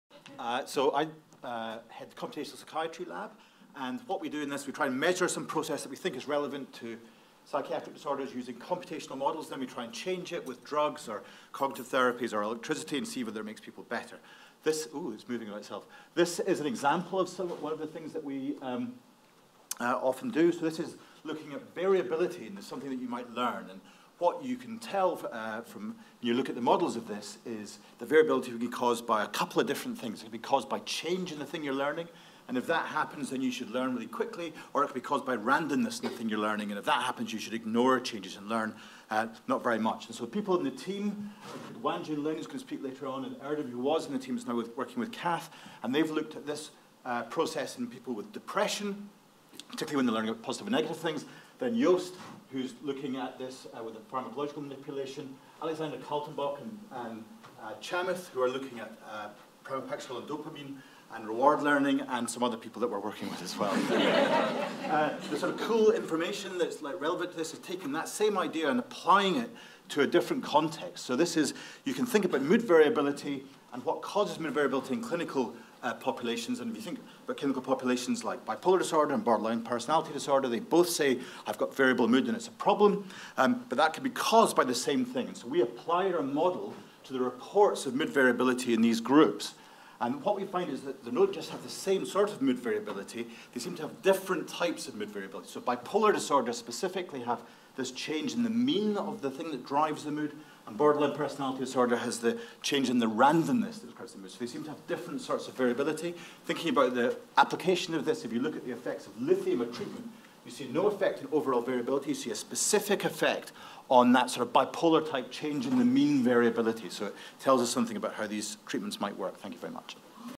Speed Round - Mike Browning: Computational Psychiatry Lab
A talk about - Computational Psychiatry Lab - presented in two minutes at a Department of Psychiatry Away Day 2020.